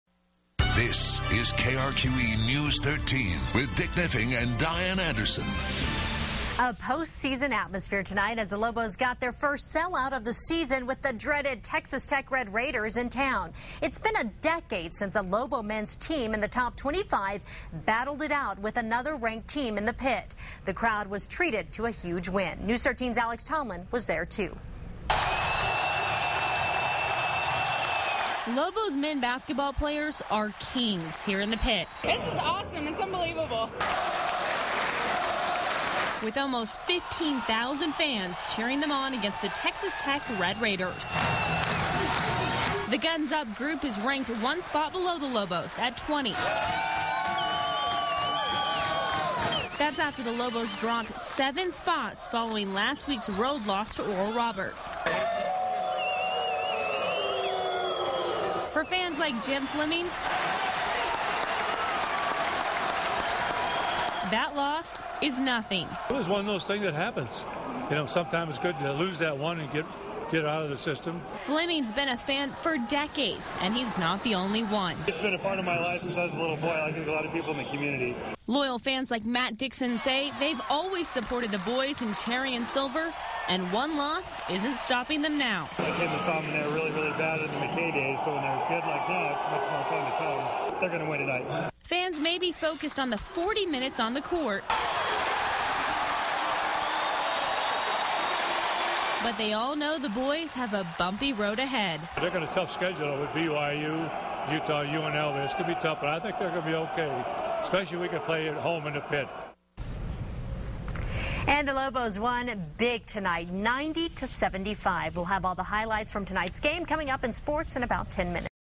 Lobo fans rabid over Pit battle
It's been a decade since a ranked Lobo men's basketball team battled a ranked opponent in the Pit, and fans in Tuesday's sellout crowd treated the victory over the dreaded Texas Tech Red Raiders as a historic win.